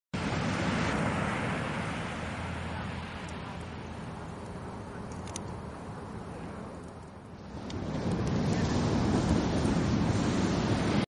Campfire Pit at Doheny Beach
There are many campfire pits at Doheny Beach, as well as many cast-iron barbecue stands.